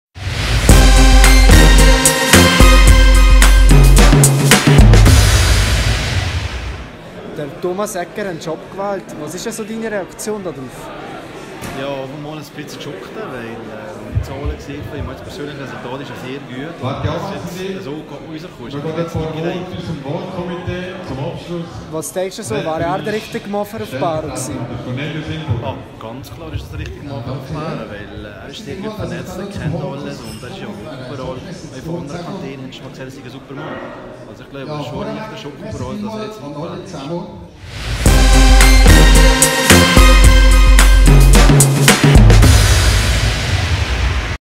Wahlen 2019 | Enzio Bregy über die Abwahl von Thomas Egger
Enzio Bregy Junge Liste CSPO über die Abwahl von Thomas Egger.